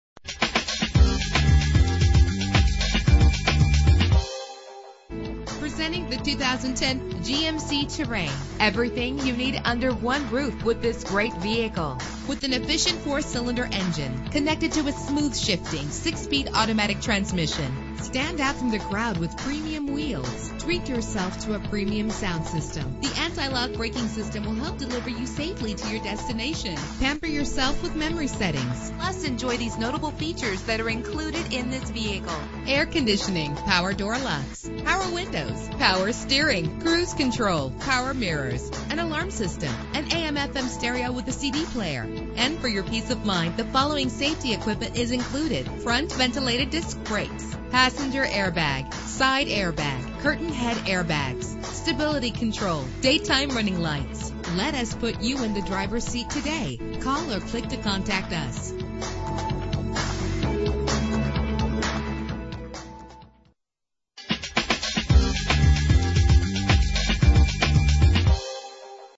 2010 GMC Terrain - Monaca PA
Year: 2010
Make: GMC
Model: Terrain
Trim: SLE
Engine: 2.4 liter inline 4 cylinder 16 valve
Transmission: 6-Speed Automatic
Color: Light Gray
Mileage: 25117
Address:
750 Beaver Valley Mall, RT18
Monaca, PA 15061